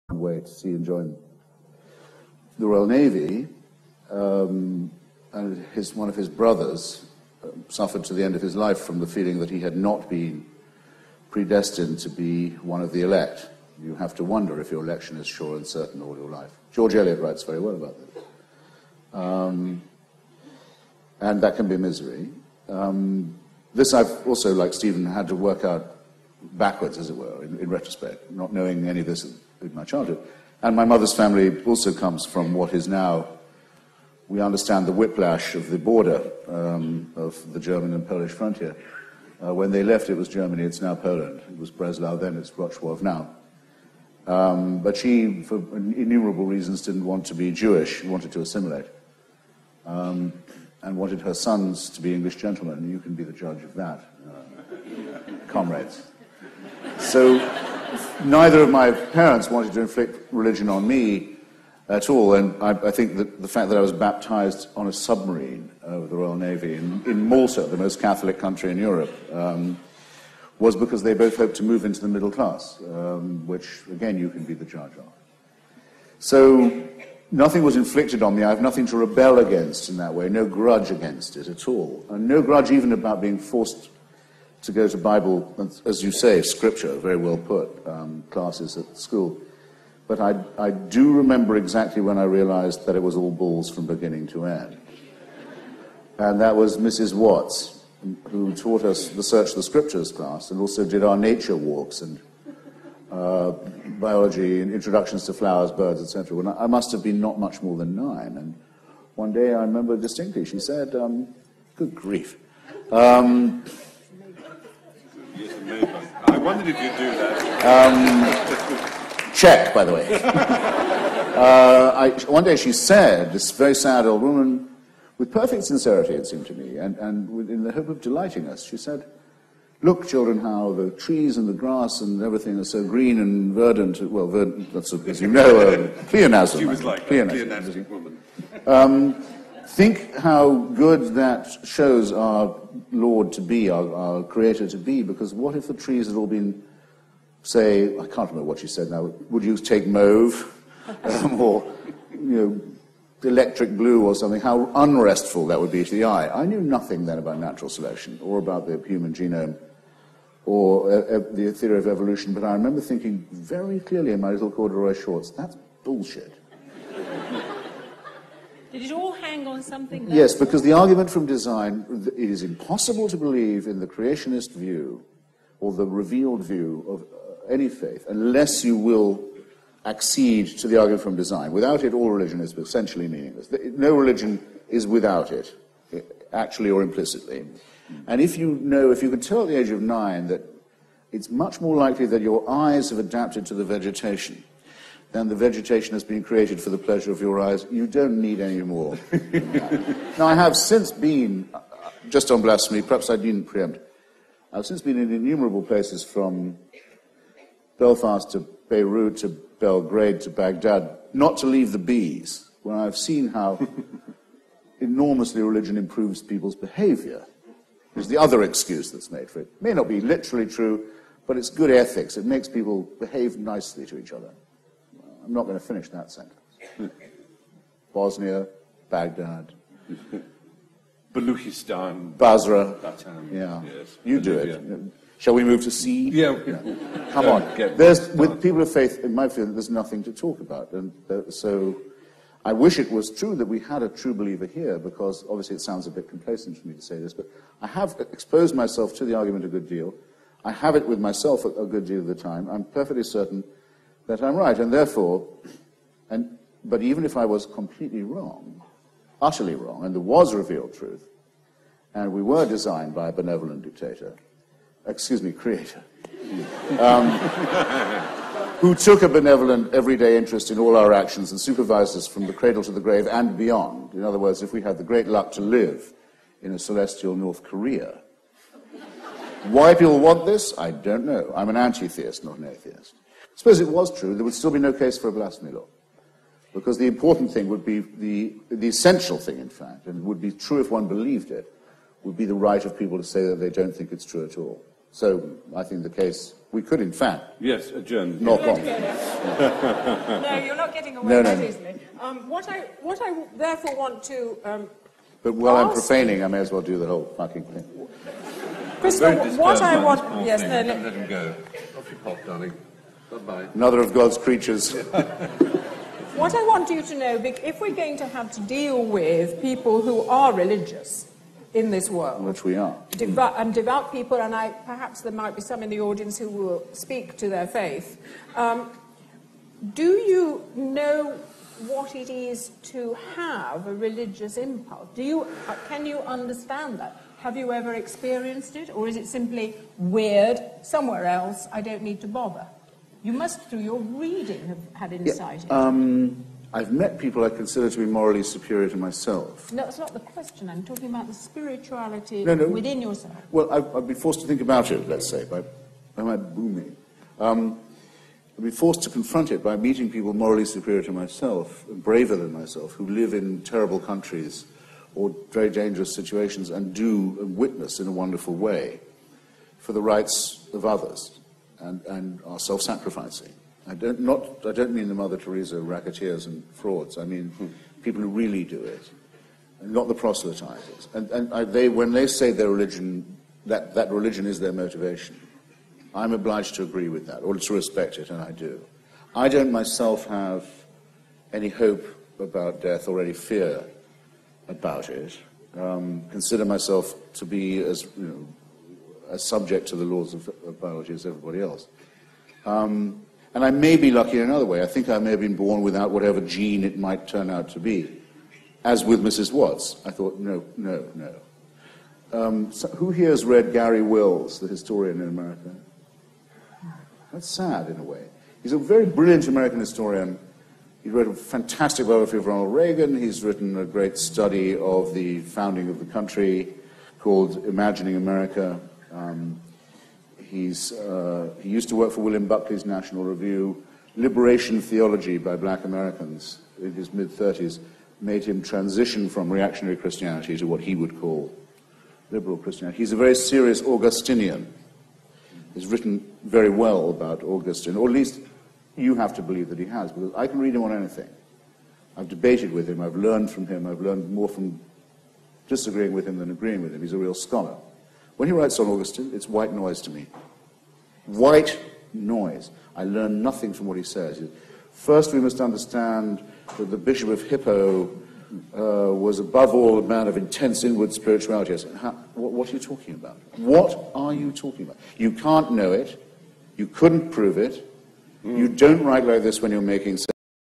Stephen Fry and Christopher Hitchens Debate Blasphemy part 2
Stephen Fry and Christopher Hitchens at the 2005 Guardian Hay Festival debating blasphemy part 2.